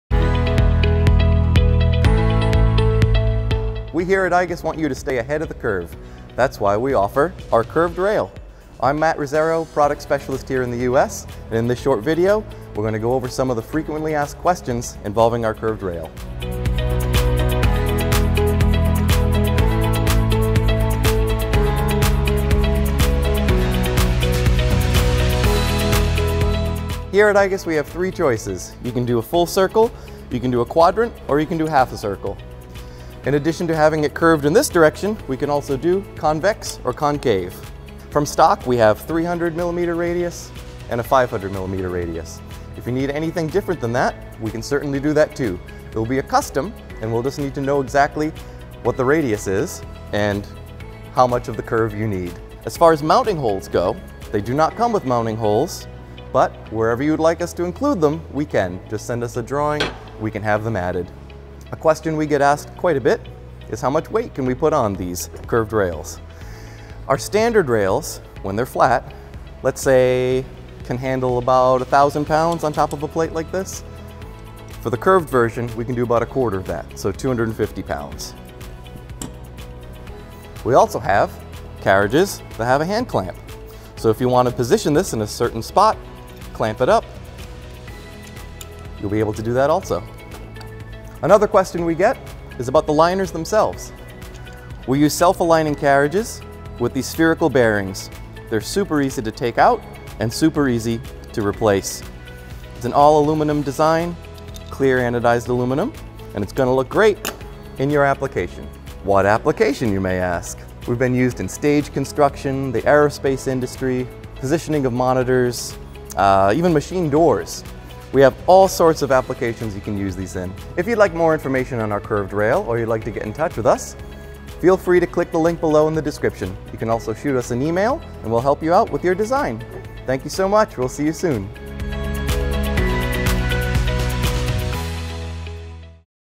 drylin® - Curved Rail FAQ
Join drylin® Product Specialist Matt Rozzero to get answers to the most frequently asked questions about our curved rails. Topics include rail choices, radius sizes, mounting holes, drylin’s® plastic liner technology, and more.